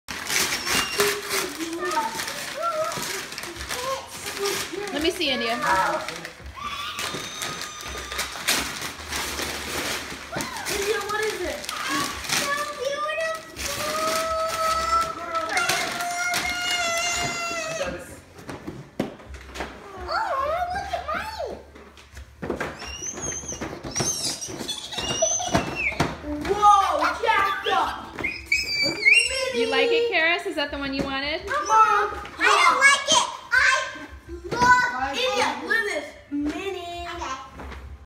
Two white girls get black dolls for Christmas. What happens next is ... actually nothing shocking h
An alternate picture of what happens when white girls are given black dolls. In response to the viral video in which two white girls pitch a fit.